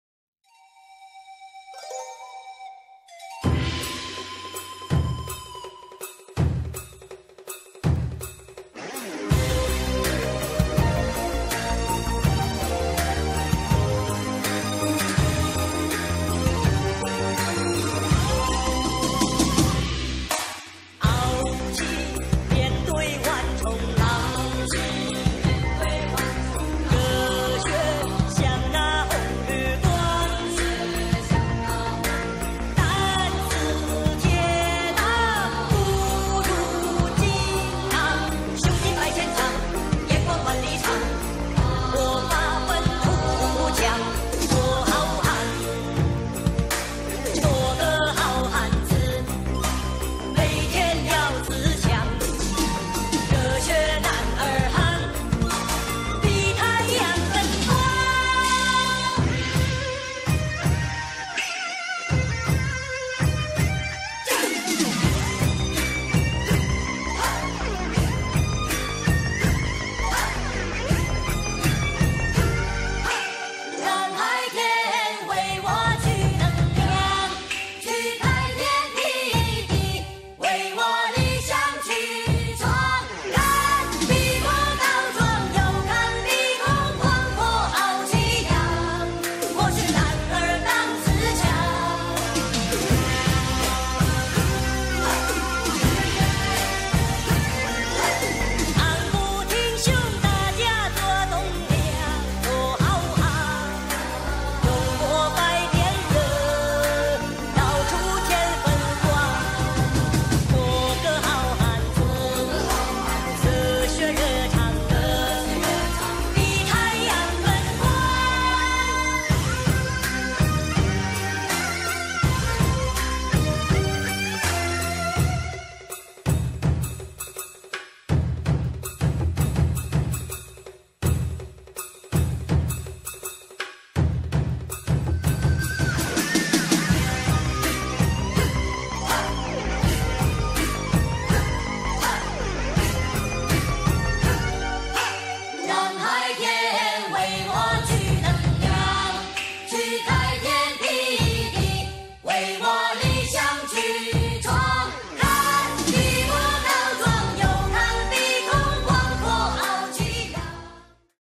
Tiger and Crane Double Paired Form- Hung Gar Kungfu
Fu Hok Seung Ying Kuen was created by Hung Hei Gwoon, having combined the Shaolin Tiger techniques with crane techniques of his wife. Wong Fei Hung is said to have re-choreographed the tiger and crane form by adding the Kiu Sau techniques learned from Tit Kiu Sarm.